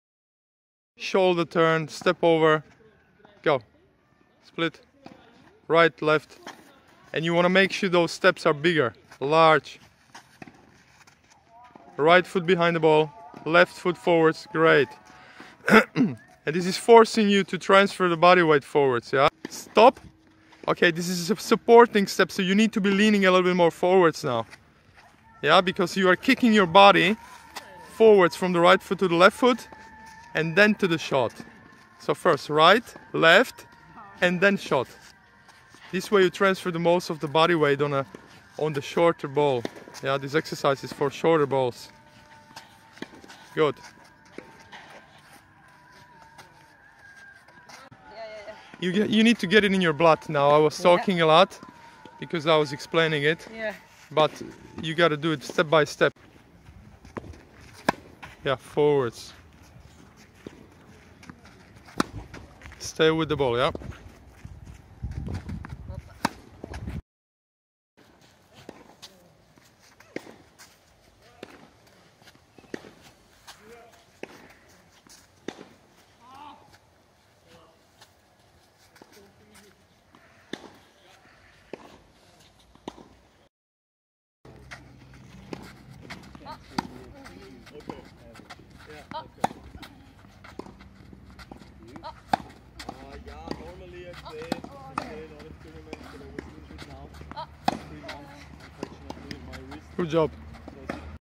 Tennis lesson: SHORT BALL'S MOVEMENT / with WTA player Dia Evtimova (BUL)
Tennis Lesson with technical expert / performance analyst Jan Solcani